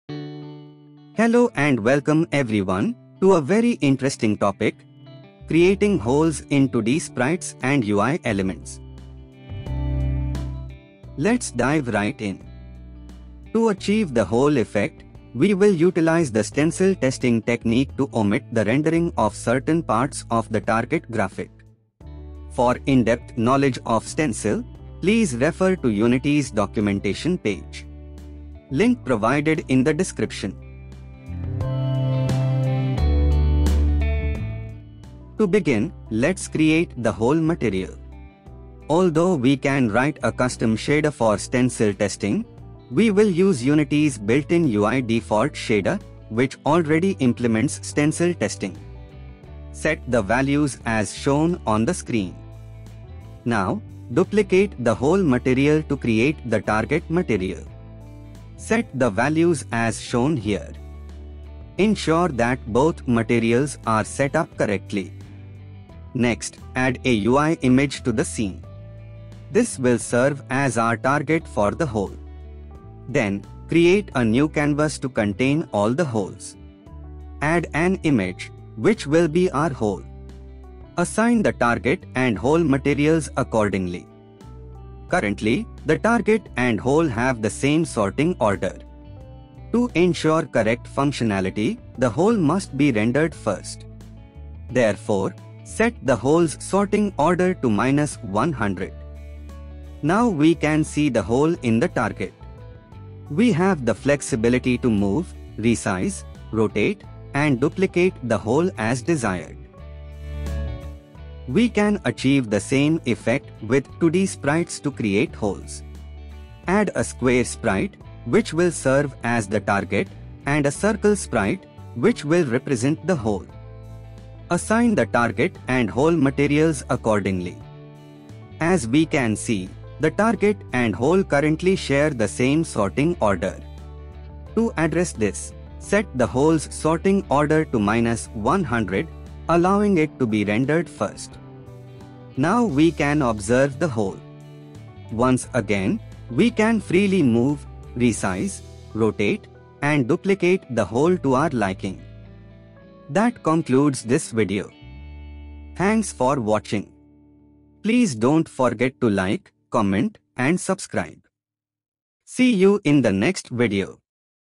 Creating Hole in 2D sprites and UI images | Stencil Testing | UI/Default Shader | Unity Game Engine
Unity's official stencil documentation-